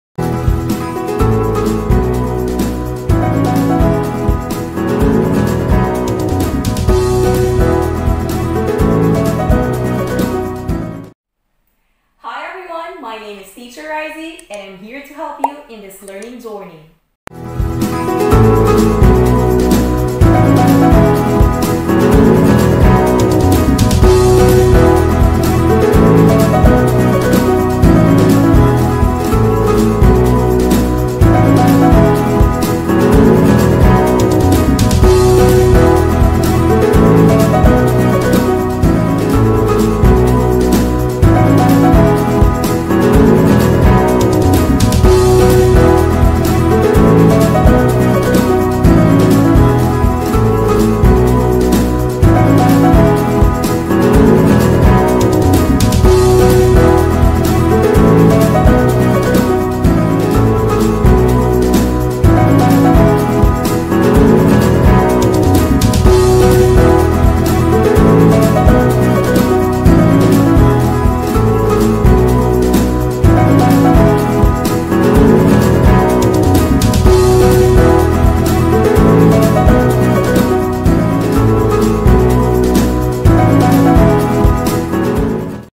WELCOME TO MY CHANNEL || Aizie Dumuk
Hi everyone! I am teacher Aizie and I create educational videos to help my students and fellow teachers. For questions and clarifications you may send me message thru my FB account
Happy learning y'all!